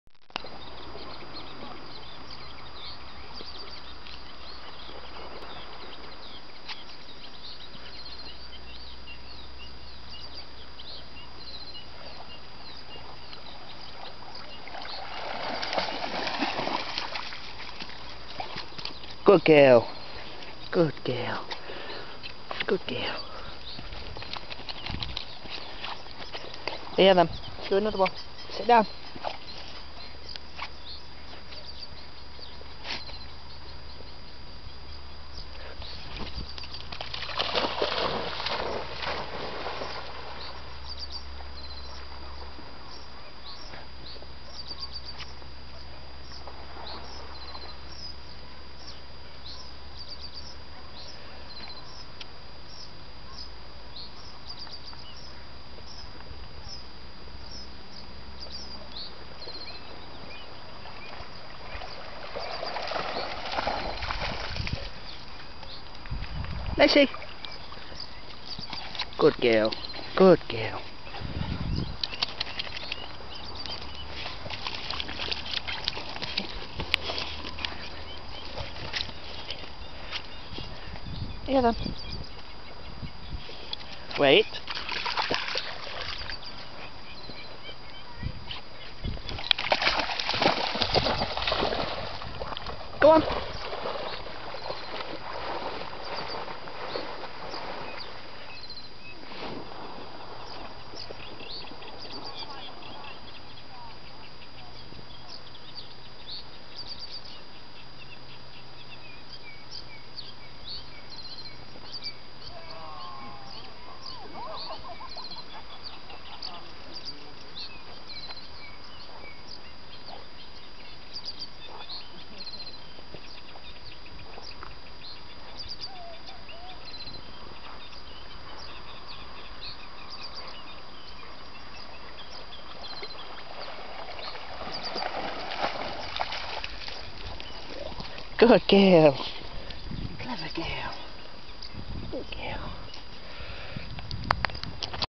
Springer spaniel retreiving tennis ball from water.AVI
So today had a nice ride out to the lake and threw a few balls :)